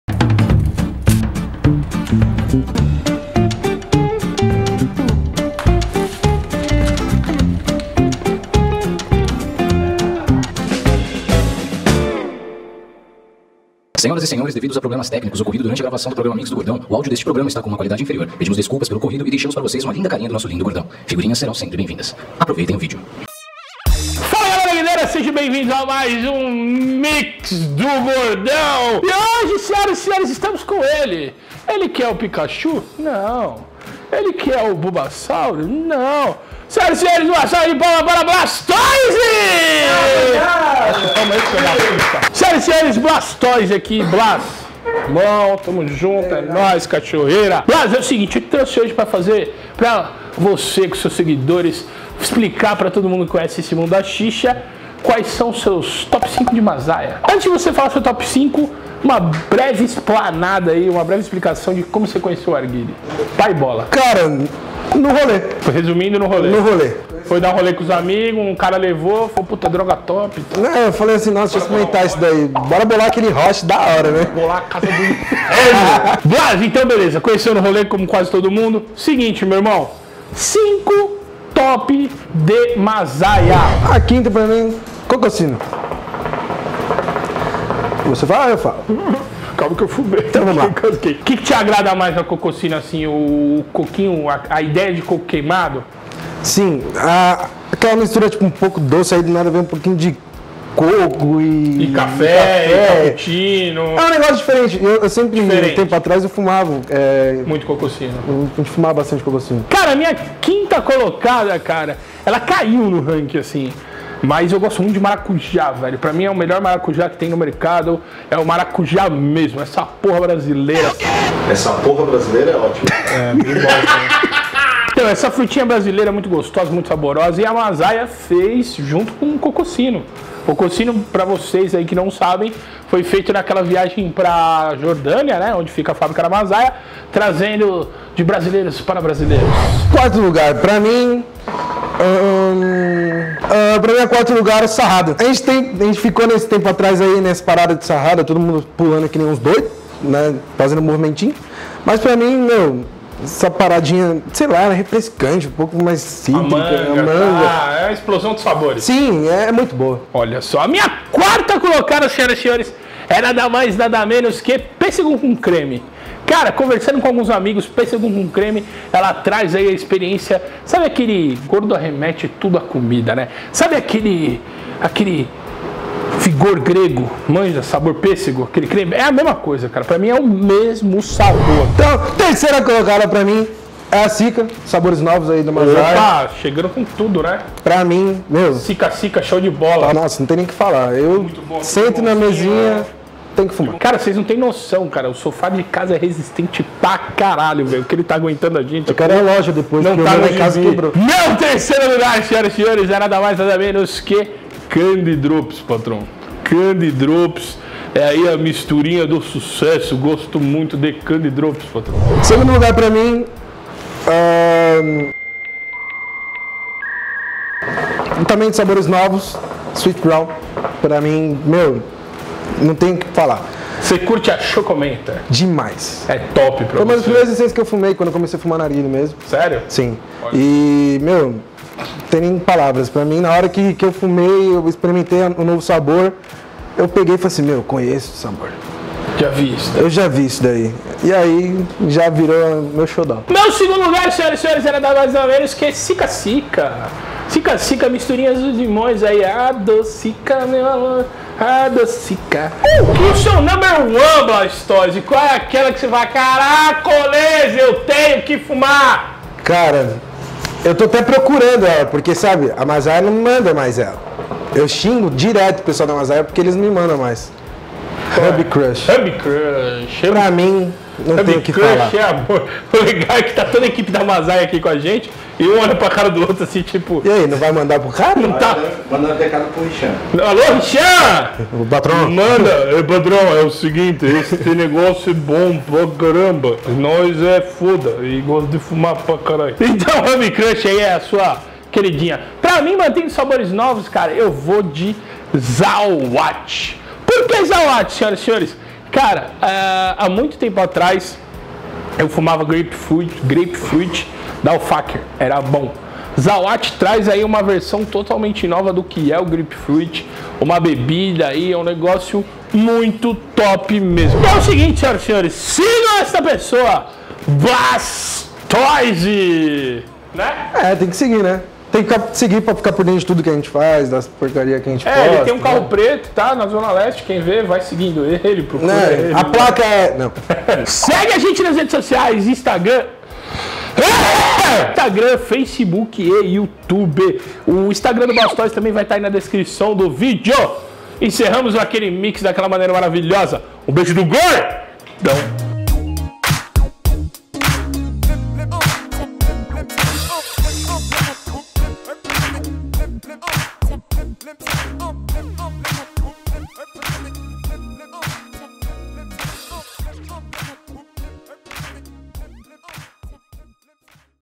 TOP 5 DE MAZAYA feat BLASTOISE l MIX DO GORDÃO [S07EP05]
FALA GALERA ARGUILEIRA!

Hoje no MIX DO GORDÃO, estamos mais uma vez sentados no sofá com o nosso querido BLASTOISE.
E vamos ver quais são as nossas TOP 5 MAZAYA.

DA O PLAY.